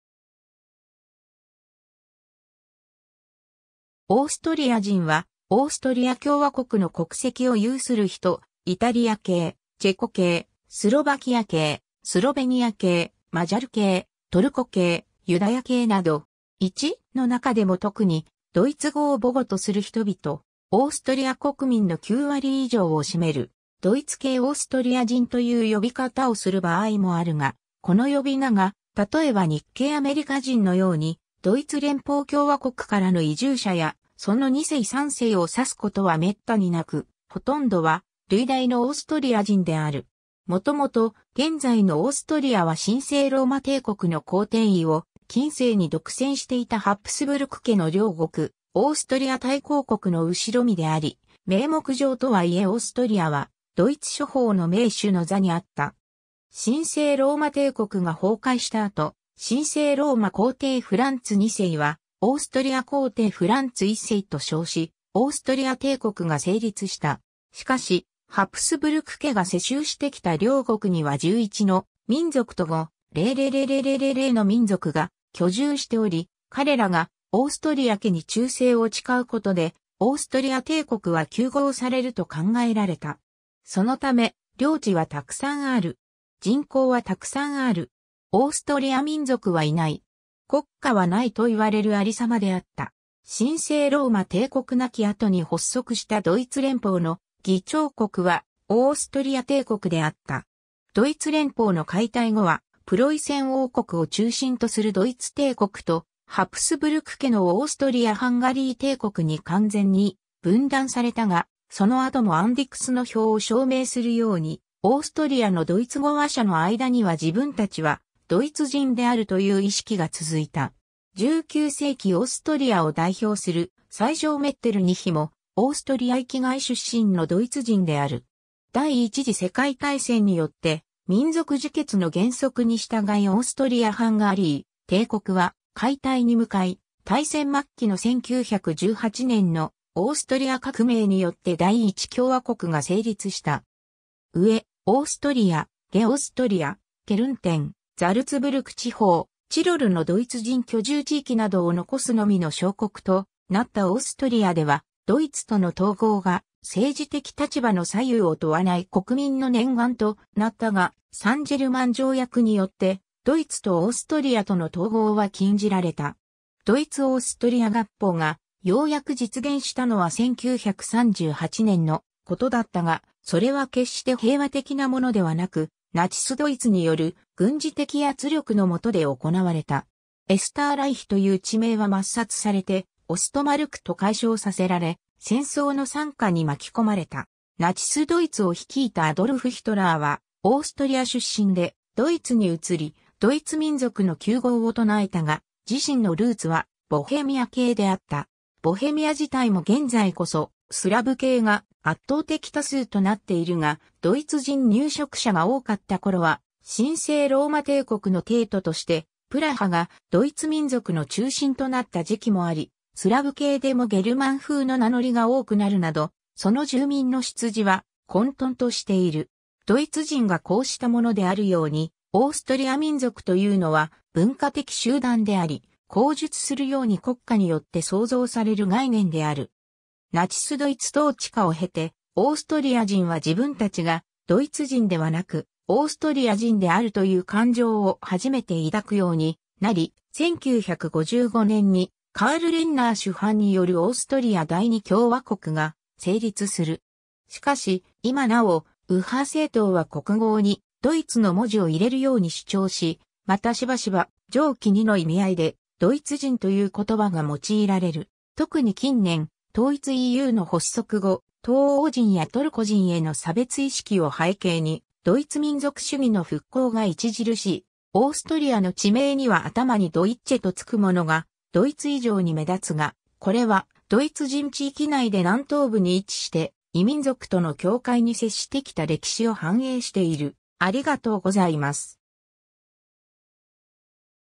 オーストリア人
オーストリア人（ドイツ語: Österreicher　女性形はÖsterreicherin）は、 オーストリア共和国の国籍を有する人（ドイツ系（バイエルン系、アレマン系の一部）、イタリア系、チェコ系、スロバキア系、スロベニア系、マジャル系、トルコ系、ユダヤ系など）。
 1.の中でも特にドイツ語（オーストリアドイツ語、バイエルン・オーストリア語、アレマン諸語の一部など）を母語とする人々。
オーストリア国民の9割以上を占める。
ドイツ系オーストリア人という呼び方をする場合もあるが、この呼び名が(たとえば日系アメリカ人のように）ドイツ連邦共和国からの移住者やその2世3世を指すことは滅多になく、ほとんどは累代のオーストリア人である。
 もともと、現在のオーストリアは神聖ローマ帝国の皇帝位を近世に独占していたハプスブルク家の領国オーストリア大公国の後身であり、名目上とはいえオーストリアはドイツ諸邦の盟主の座にあった。
神聖ローマ帝国が崩壊した後、神聖ローマ皇帝フランツ2世はオーストリア皇帝フランツ1世と称し、オーストリア帝国が成立した。
しかしハプスブルク家が世襲してきた領国（ハプスブルク帝国）には11の民族と5,000万の民族が居住しており、彼らがオーストリア家（ハプスブルク家）に忠誠を誓うことでオーストリア帝国は糾合されると考えられた。
そのため「領地はたくさんある。人口はたくさんある。オーストリア民族はいない。国家はない」（H・アンディクス）と言われる有様であった。
神聖ローマ帝国亡き後に発足したドイツ連邦の議長国はオーストリア帝国であった。
ドイツ連邦の解体後はプロイセン王国を中心とするドイツ帝国とハプスブルク家のオーストリア＝ハンガリー帝国に完全に分断されたが、その後もアンディクスの評を証明するように、オーストリアのドイツ語話者の間には自分たちは「ドイツ人」であるという意識が続いた。
19世紀オーストリアを代表する宰相メッテルニヒも、オーストリア域外出身のドイツ人である。
 第一次世界大戦によって「民族自決の原則」に従いオーストリア＝ハンガリー帝国は解体に向かい、大戦末期の1918年のオーストリア革命によって第一共和国が成立した。
上オーストリア、下オーストリア、ケルンテン、ザルツブルク地方、チロルのドイツ人居住地域（南チロルを除く）などを残すのみの小国となったオーストリアでは、ドイツとの統合が政治的立場の左右を問わない国民の念願となったが、サン＝ジェルマン条約によってドイツとオーストリアとの統合は禁じられた。
 ドイツ・オーストリア合邦がようやく実現したのは1938年のことだったが、それは決して平和的なものではなく、ナチス・ドイツによる軍事的圧力のもとで行われた。
エスターライヒ（オーストリアのドイツ語名）という地名は抹殺されて「オストマルク」と改称させられ、戦争の惨禍に巻き込まれた。
ナチス・ドイツを率いたアドルフ・ヒトラーはオーストリア出身で、ドイツに移り「ドイツ民族の糾合」を唱えたが、自身のルーツはボヘミア（チェコ）系（ボヘミア・ドイツ人?）であった。
ボヘミア自体も現在こそスラヴ系が圧倒的多数となっているが、ドイツ人入植者が多かったころは神聖ローマ帝国の帝都としてプラハがドイツ民族の中心となった時期もあり、スラヴ系でもゲルマン風の名乗りが多くなるなど、その住民の出自は混沌としている。
ドイツ人がこうしたものであるように、オーストリア民族というのは文化的集団であり、後述するように国家によって創造される概念である。
 ナチス・ドイツ統治下を経て、オーストリア人は自分たちがドイツ人ではなくオーストリア人であるという感情（アイデンティティ）を初めて抱くようになり、1955年にカール・レンナー首班によるオーストリア第二共和国が成立する。
しかし、今なお右派政党は国号にドイツの文字を入れるように主張し、またしばしば、上記2の意味合いでドイツ人という言葉が用いられる。
特に近年、統一EUの発足後、東欧人やトルコ人への差別意識を背景にドイツ民族主義の復興が著しい。
オーストリアの地名には頭にドイッチェ（ドイツ人の）とつくものがドイツ以上に目立つが、これはドイツ人地域内で南東部に位置して異民族との境界に接してきた歴史を反映している。